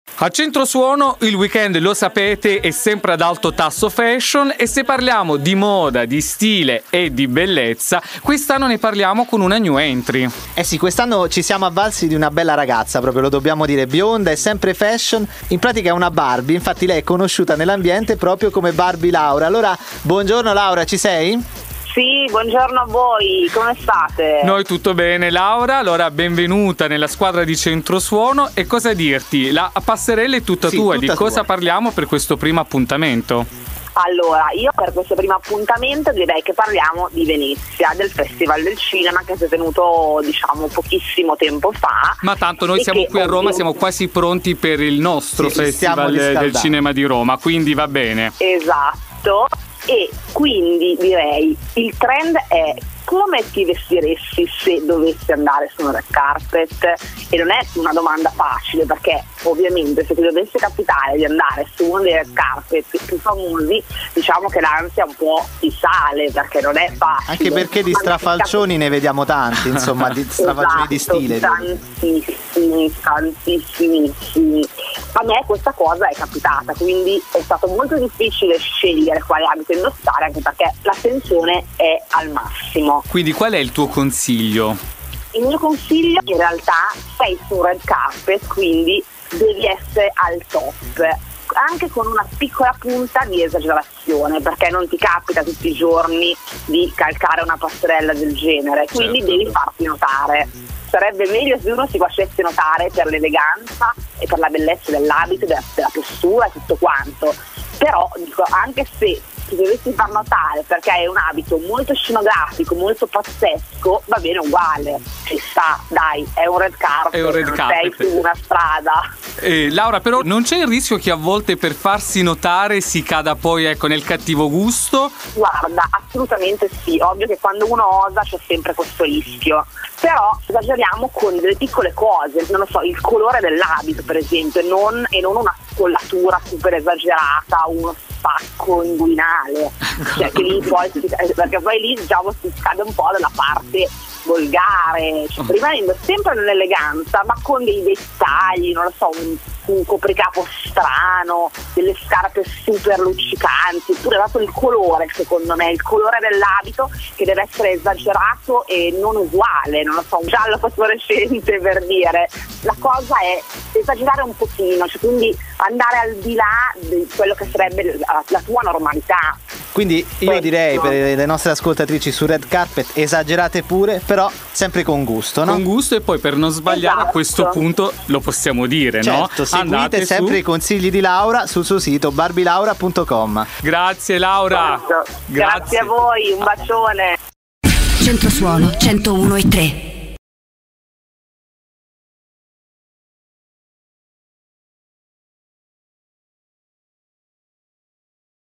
Centro Suono Style - Barbie Laura
La moda al femminile vista dalla nostra fashion blogger, Barbie Laura. In questo weekend parliamo di come affrontare la sfida del red carpet. Tutti i consigli di Laura su